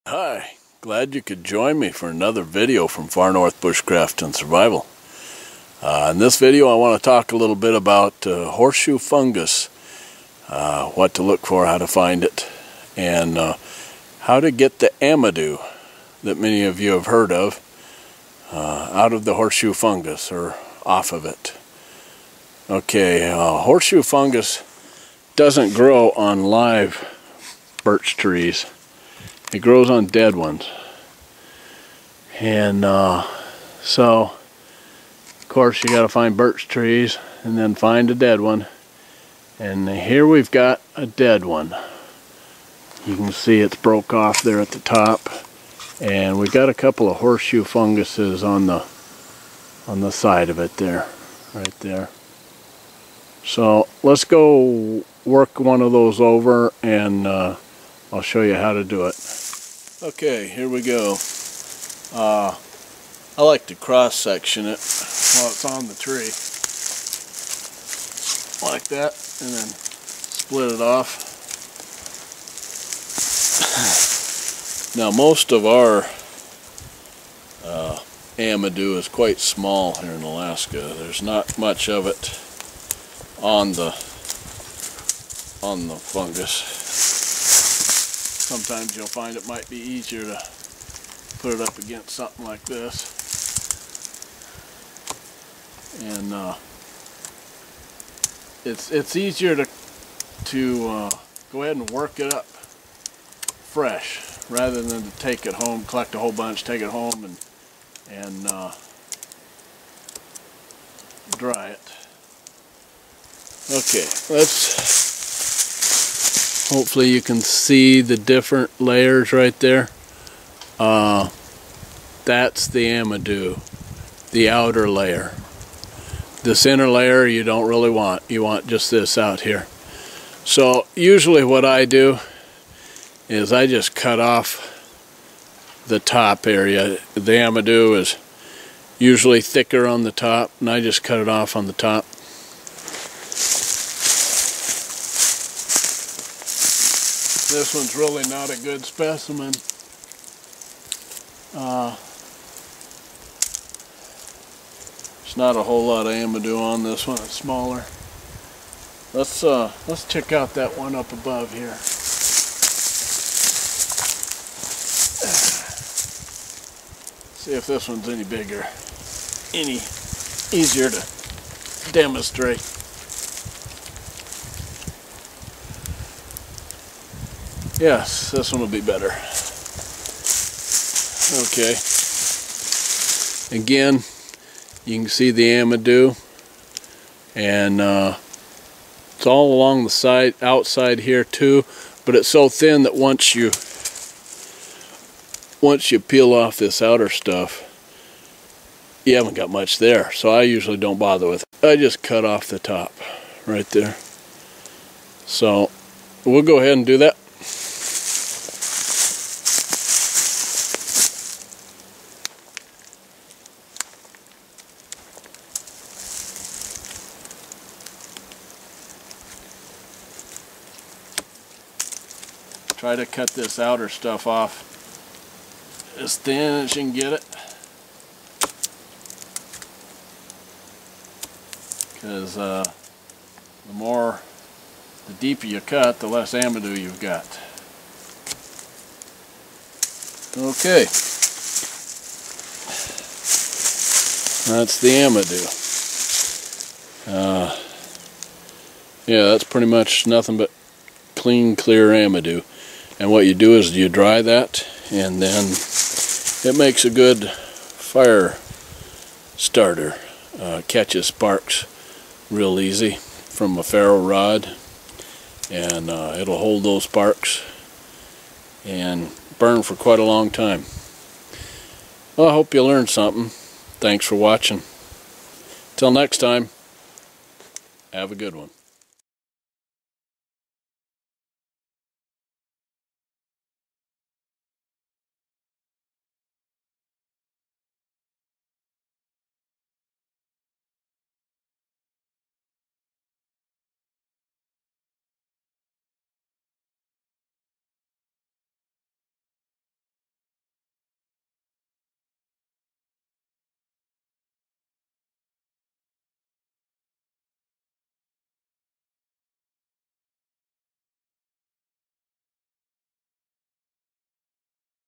Horse Hoof Fungus - Amadou Tips
Lonnie shows a few tips on what to look for and where to find Horses Hoof Fungus and the Amadou that is part of the fungus.   Lonnie also shows how to separate the Amadou from the rest of the fungus.